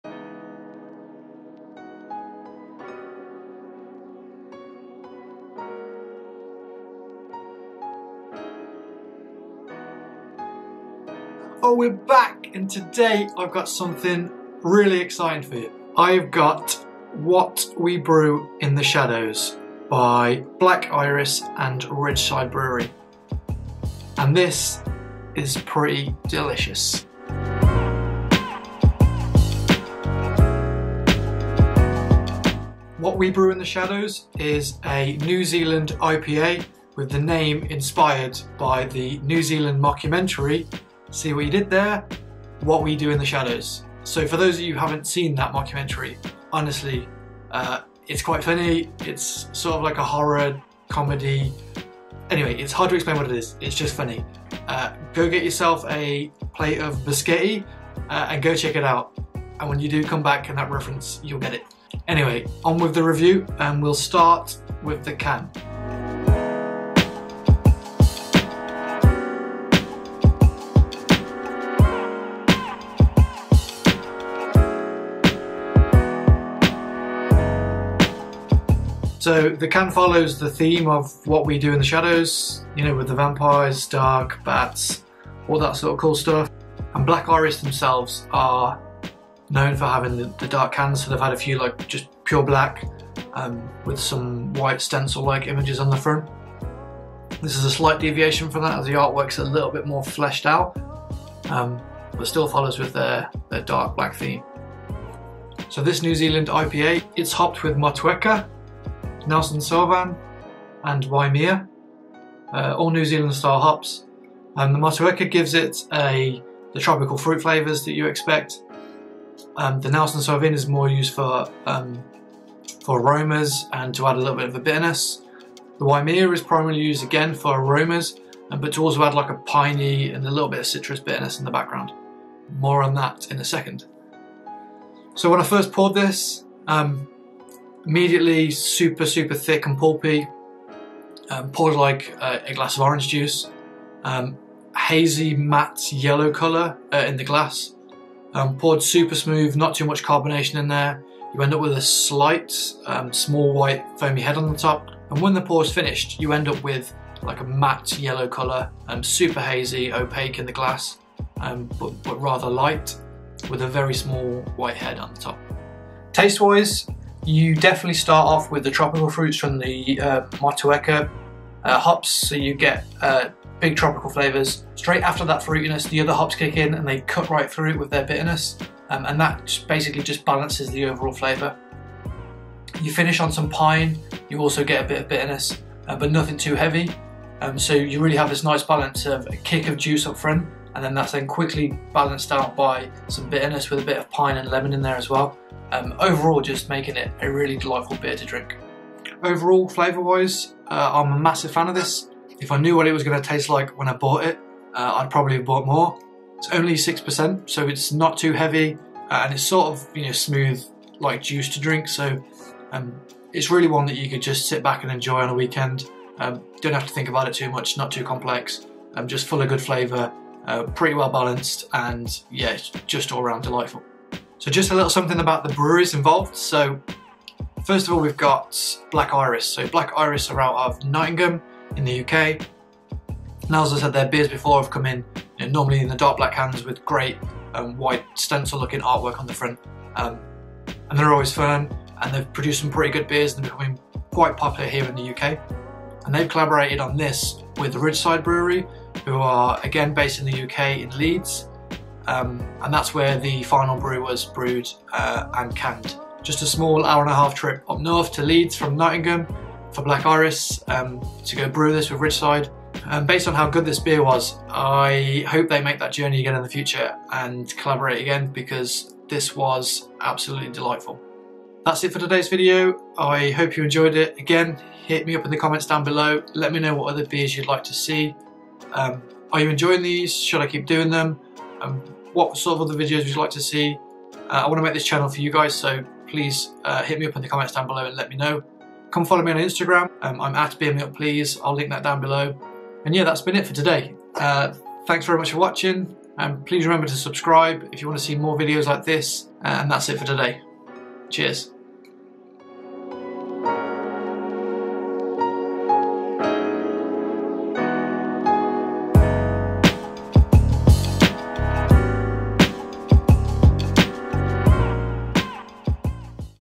Craft Beer Review | What We Brew in the Shadows | NZIPA | Black Iris x Ridgeside Brewery
A review of the Black Iris and Ridgeside brewery collaboration: What We Brew in the Shadows, a hazy New England IPA (NEIPA).